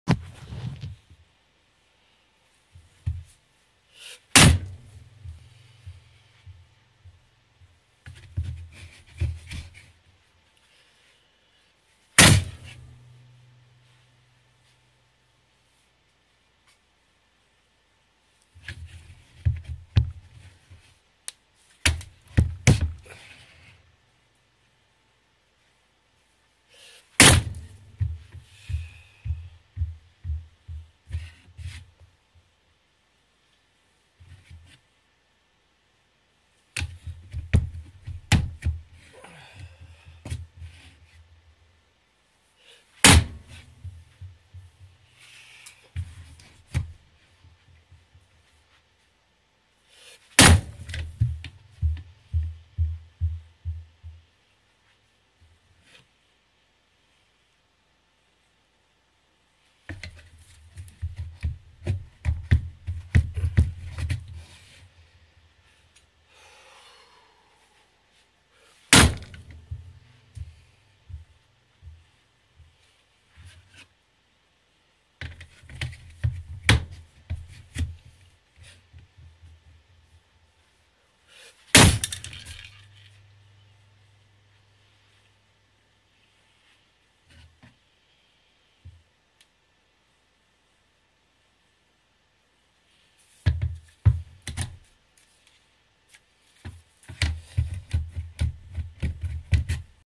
Skallywag Tactical dagger vs. Serpa holster
Short video of some torture testing we're doing on the Skallywag Tactical dagger.